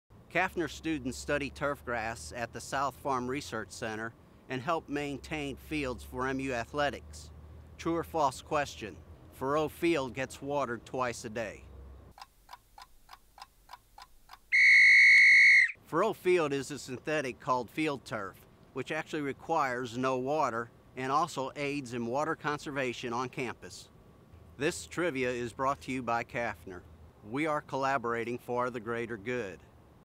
Turf at Memorial Stadium
Brad Fresenburg, assistant extension professor, plant sciences, discussing the turf used at Memorial Stadium.